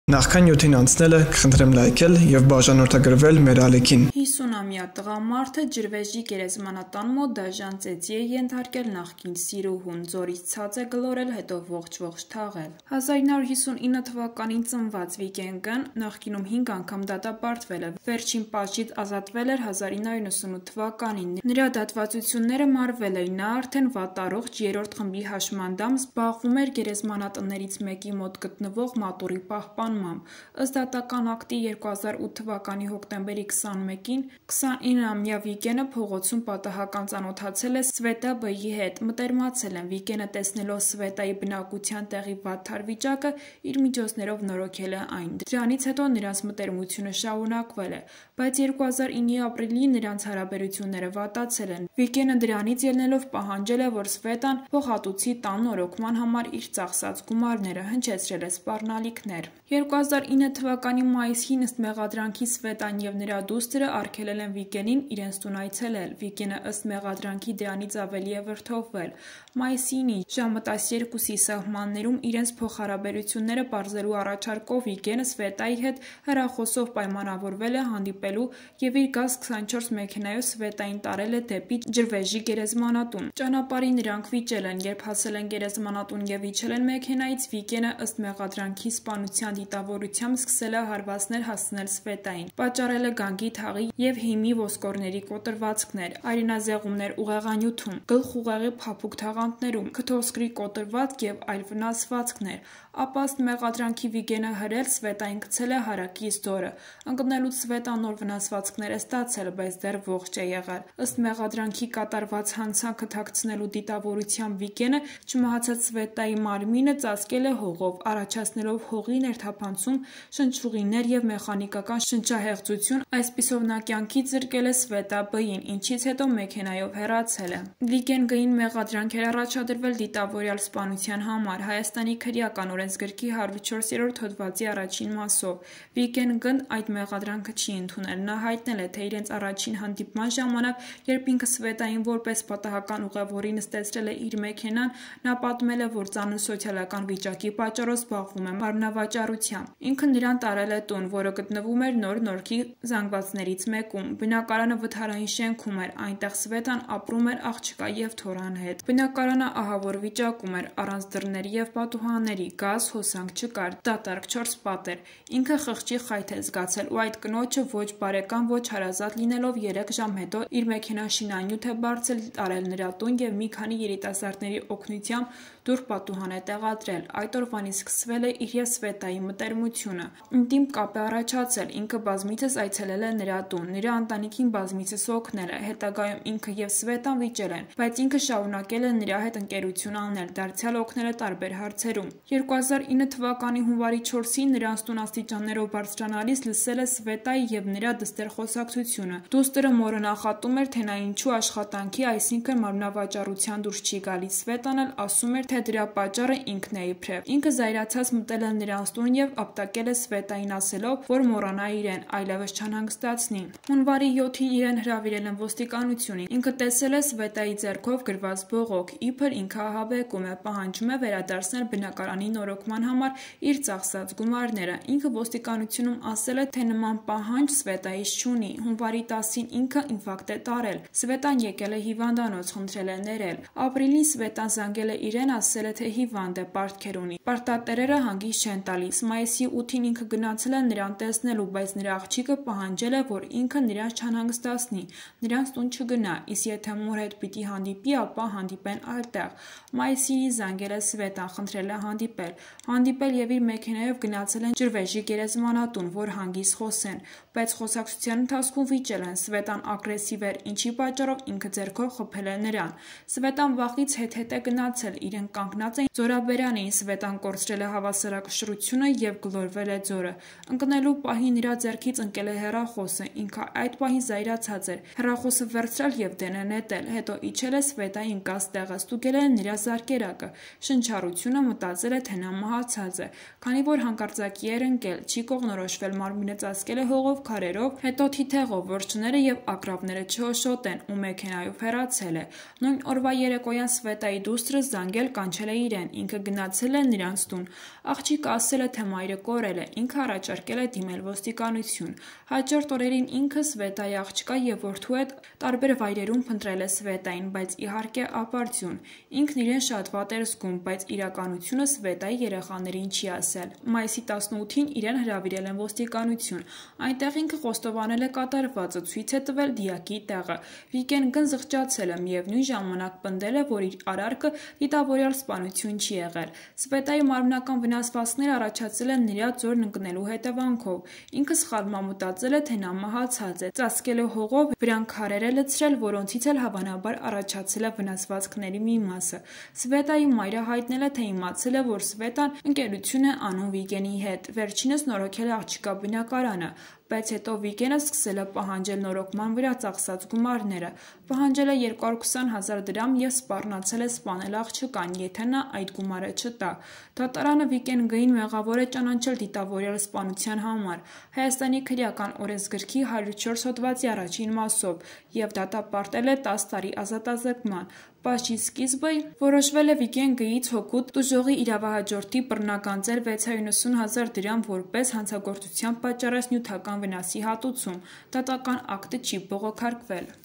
50-ամյա տղամարդը դաժան ծեծի է ենթարկել նախկին սիրուհուն, ձորից ցած է գլորել, հետո ողջ-ողջ թաղել
50-ամյա տղամարդը Ջրվեժի գերեզմանատան մոտ դաժան ծեծի է ենթարկել նախկին սիրուհուն, ձորից ցած է գլորել, հետո ողջ-ողջ թաղել